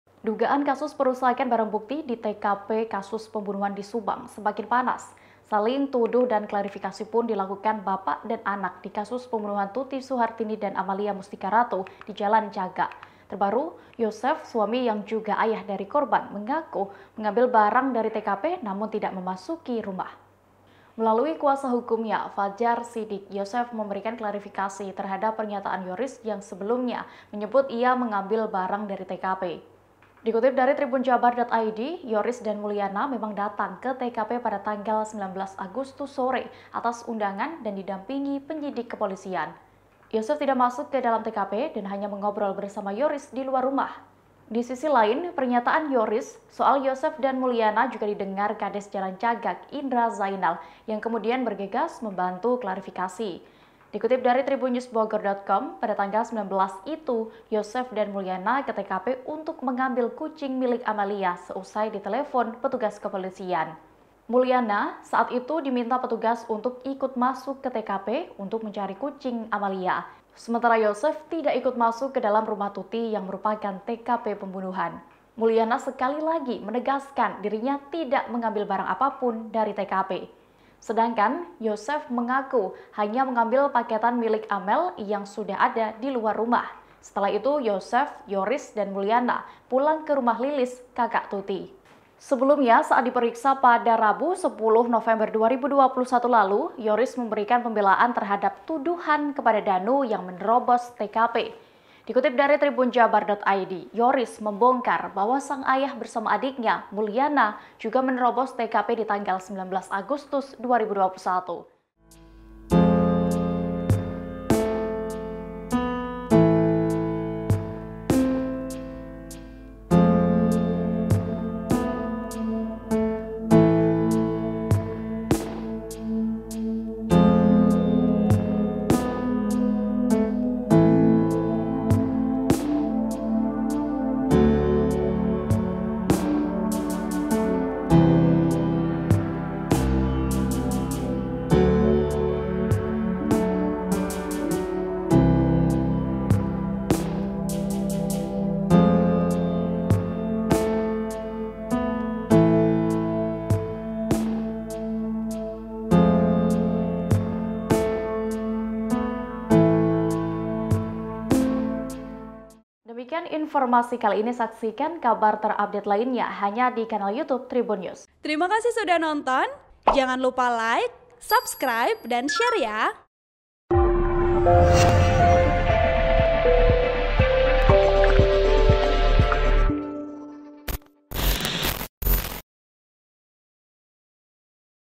Yosef Akui Ambil Barang Milik Amalia Tapi Bantah Masuk Rumah di Subang, Begini Pengakuan Lengkapnya
TRIBUN-VIDEO.COM - Dugaan kasus perusakan barang bukti di TKP kasus Subang semakin panas.

Saling tuduh dan klarifikasi pun dilakukan bapak dan anak di kasus pembunuhan Tuti Suhartini dan Amalia Mustika Ratu di Jalancagak.

Terbaru, Yosef, suami yang juga ayah dari korban mengaku mengambil barang dari TKP namun tidak memasuki rumah.